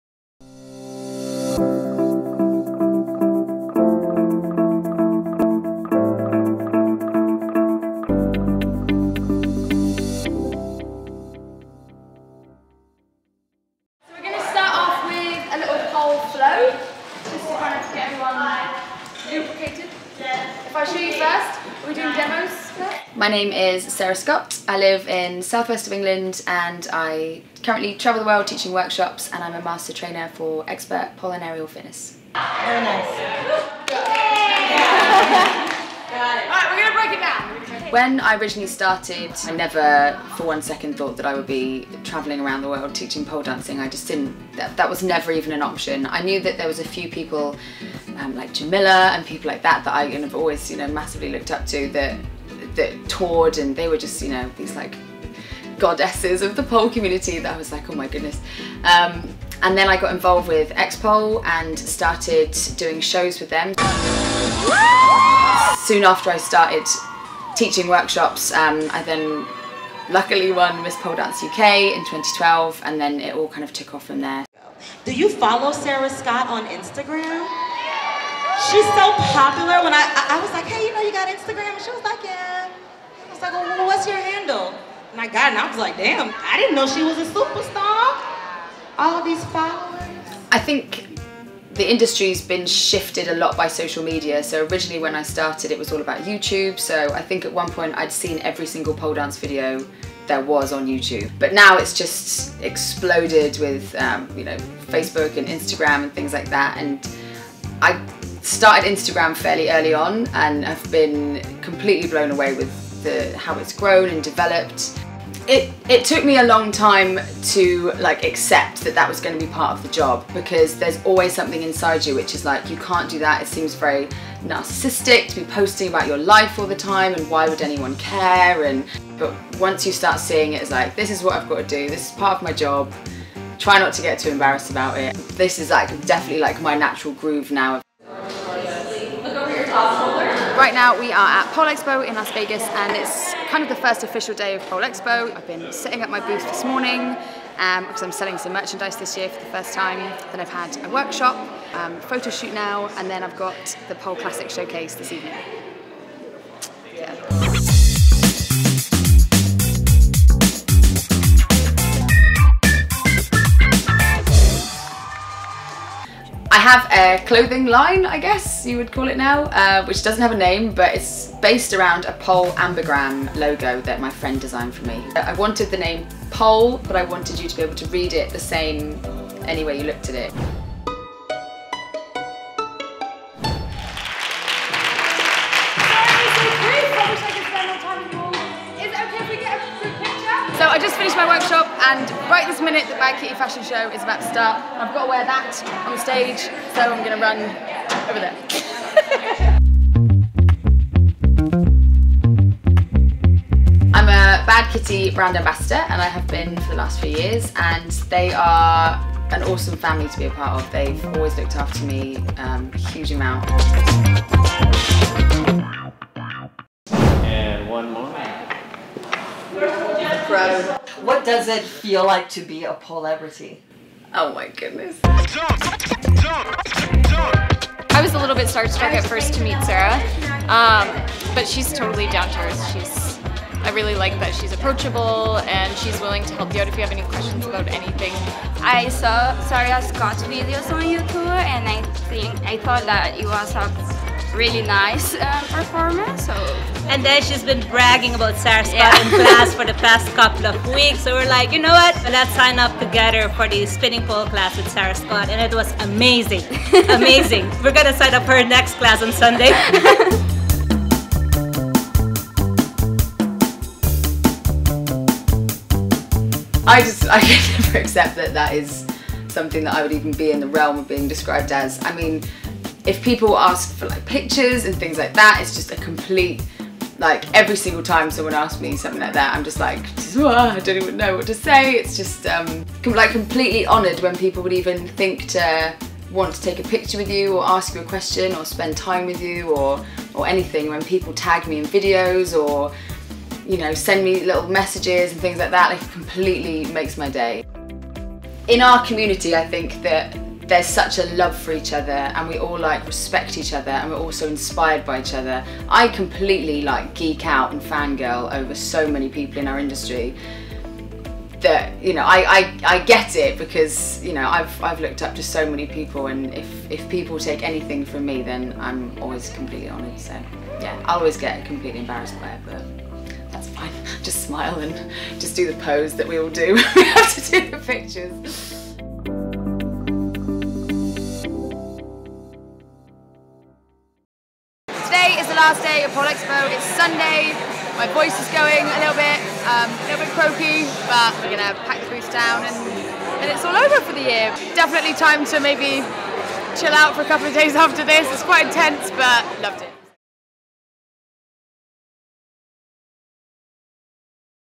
The Pole Dancing Chronicles - Season 1 Episode 6: Sarah Scott
Meet Sarah Scott! This bombshell is a pole fitness model, Bad Kitty Brand Ambassador and Master Trainer for XPERT. Sarah has been performing and instructing for the last seven years and now has her own clothing line and Off The Pole online training series.  We caught up with Sarah at the 2016 Pole Expo to find out what it's like to be a globetrotting pole instructor and performer and to learn som fun facts!